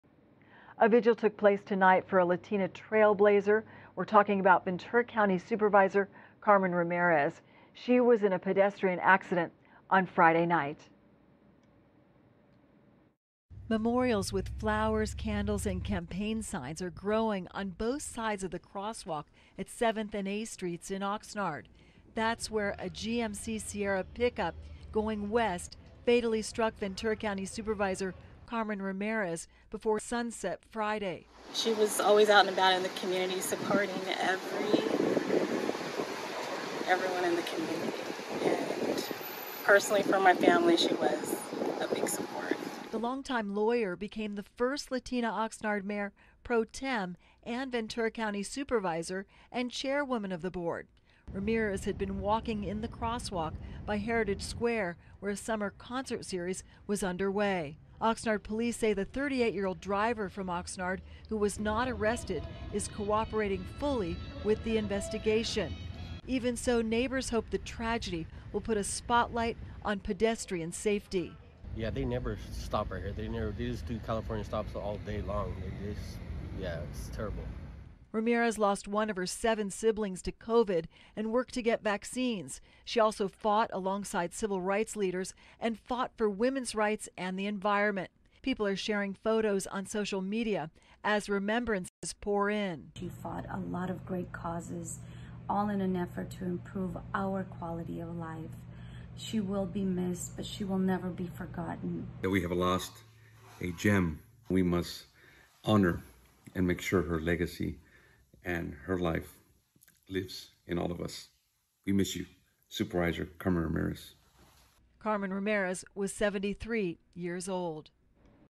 Ventura County Supervisor Carmen Ramirez dies from injuries after pedestrian-involved car ...
Ventura County Supervisor Carmen Ramirez dies from injuries after pedestrian-involved car crash [(819508)]